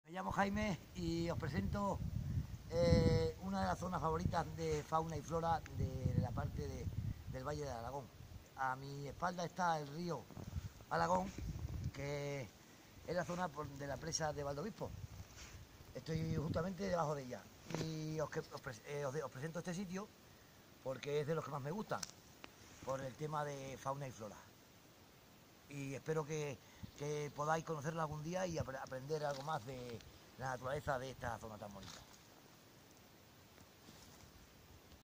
Mi rincón favorito: el río Alagón - Jaime
Jaime nos presenta un entrañable rincón del río Alagón a su paso por la presa de Valdeobispo.